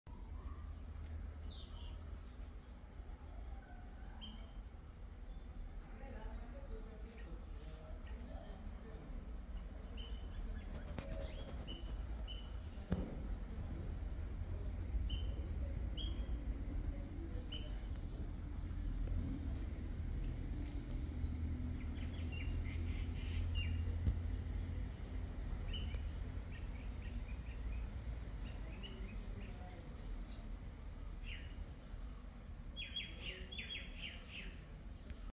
Galaxy S23 Ultra 8K Video Sample - Unedited (8K 30FPS)
Phone: S23 Ultra
Camera: Main camera (8K can only be recorded with main camera)
Duration: 35 seconds
File Size: 338 MB
Format: Mp4